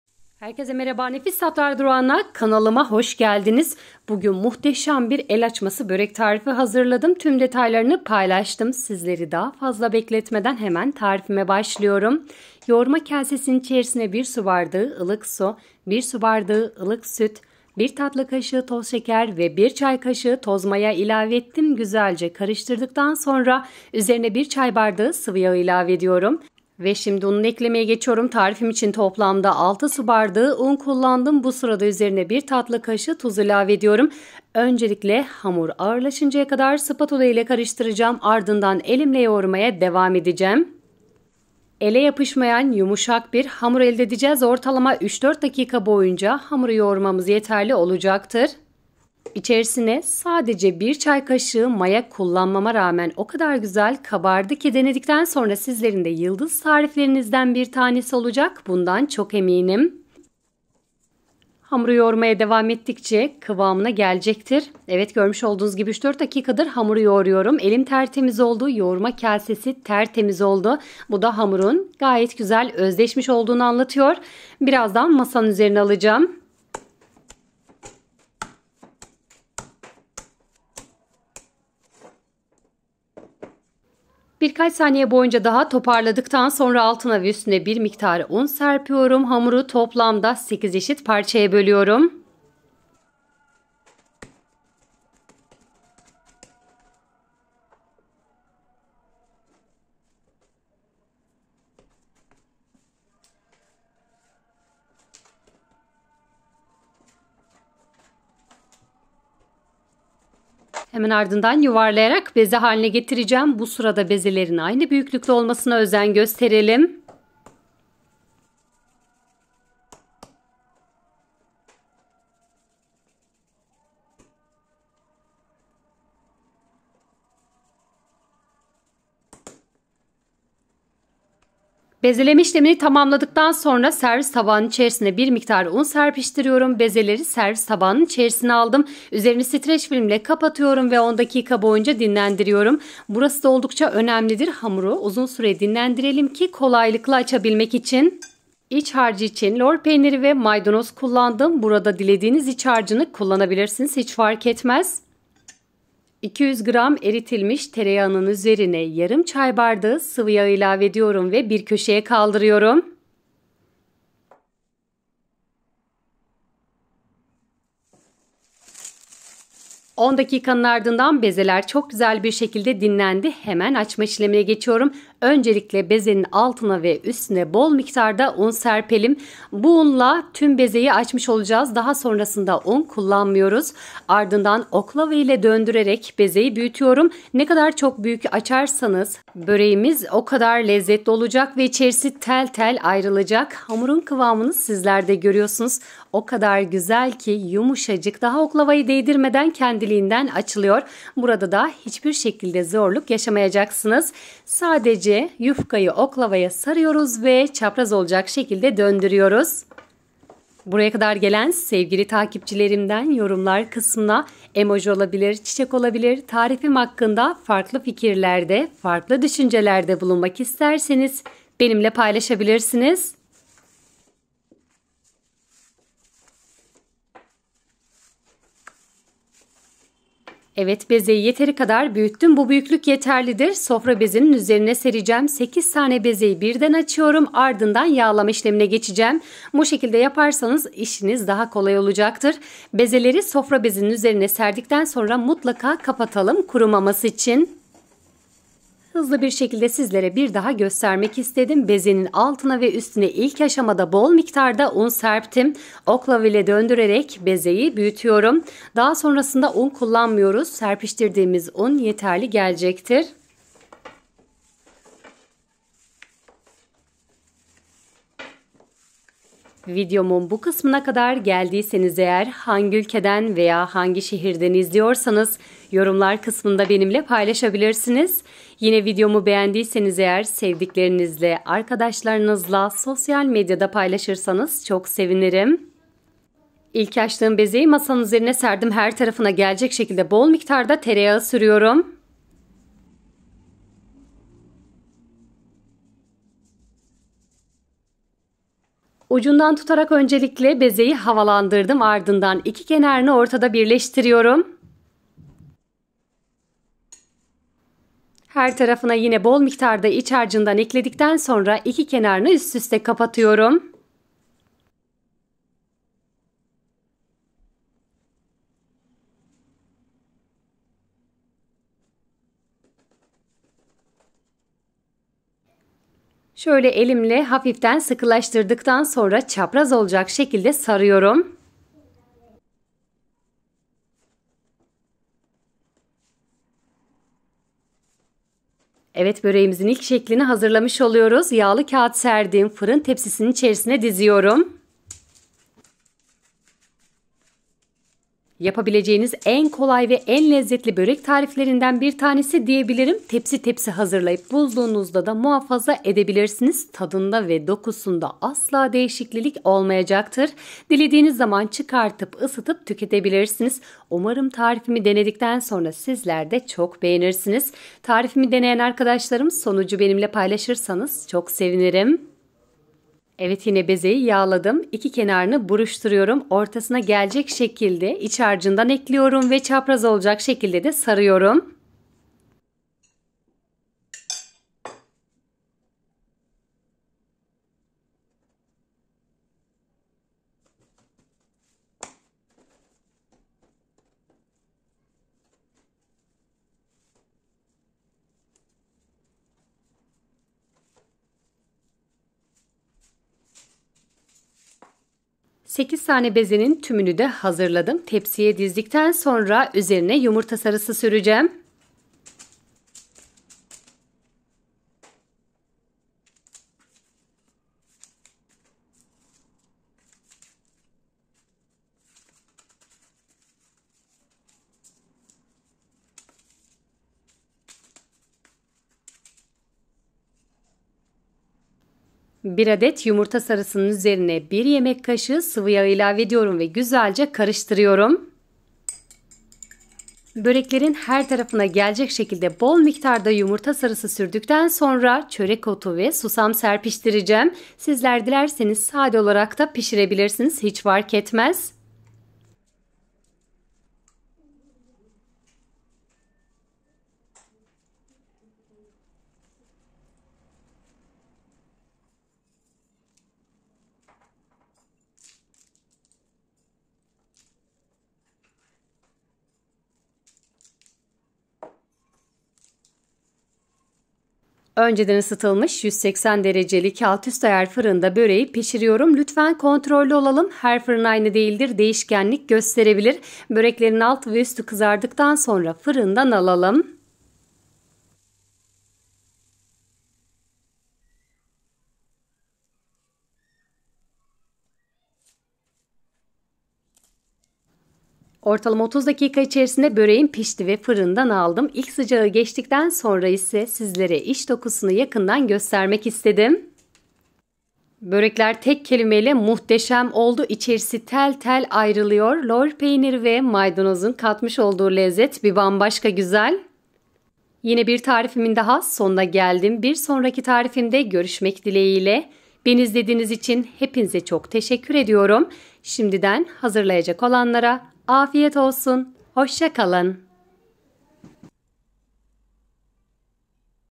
BU TARİFLE EL AÇMASI BÖREK YAPMAYAN KALMASIN!! ÇITIR ÇITIR YOK BÖYLE BİR LEZZET💯KOLAY BÖREK
Tarif İçin Gerekli Malzemeler:

1 su bardağı su
1 su bardağı süt
1 çay bardağı sıvıyağ
1 tatlı kaşığı toz şeker
1 çay kaşığı kuru maya
5.5 su bardağı un
1 tatlı kaşığı tuz

lor peyniri
maydonoz

Eritilmiş tereyağı (200 gr).
yarım çay bardağı sıvıyağ
1 adet yumurta sarısı.
1 çay kaşığı susam.
1 çay kaşığı çörek otu.

afiyet olsun.

Ingredients Required for the Recipe:

1 glass of water
1 glass of water Milk
1 teacup of oil
1 teaspoon of granulated sugar
1 teaspoon of dry yeast
5.5 cups flour
1 teaspoon salt

For internal mortar;
curd cheese
parsley

Melted butter (200 g).
half a teaspoon of oil
1 egg yolk.
1 teaspoon sesame.
1 teaspoon of nutmeg.